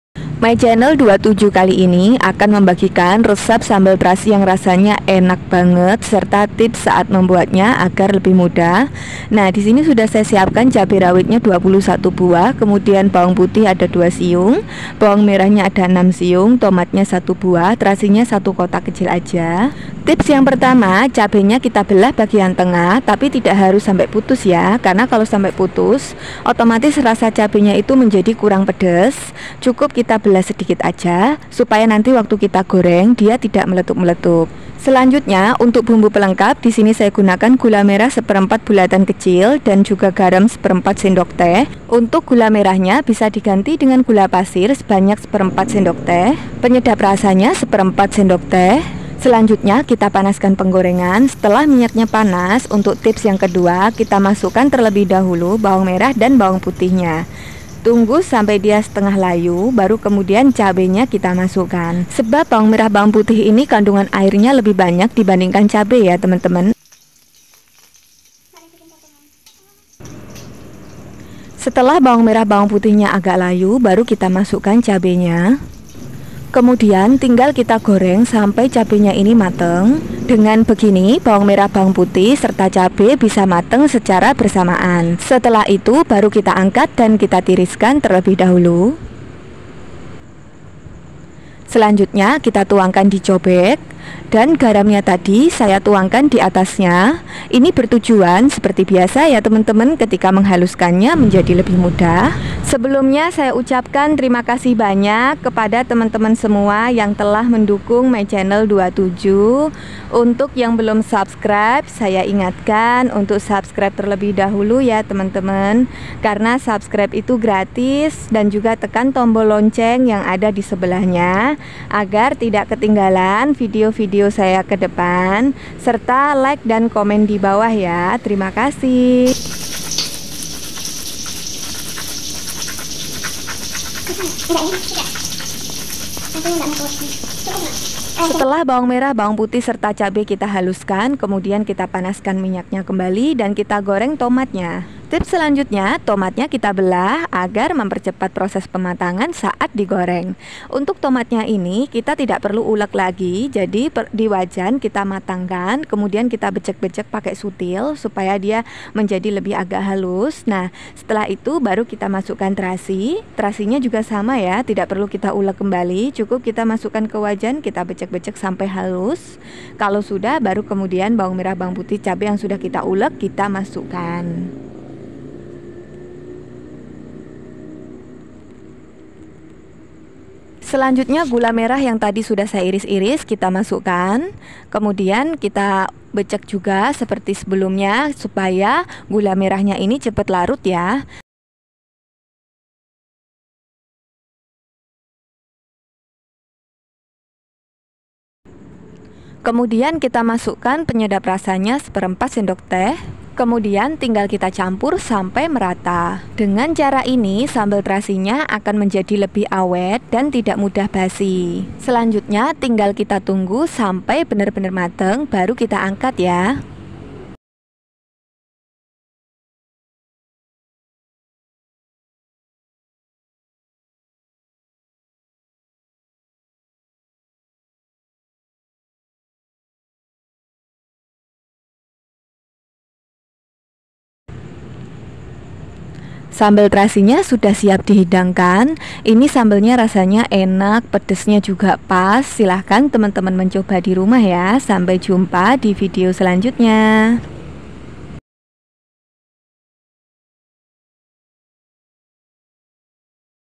Ini Tips nya! Resep sambal terasi enak dan tahan lama
@MayChanel27 akan membagikan resep sambal terasi yang enak dan tahan lama , serta tips saat memasak agar lebih mudah , ikuti vidio nya sampai selesai , agar tidak sampai salah ya.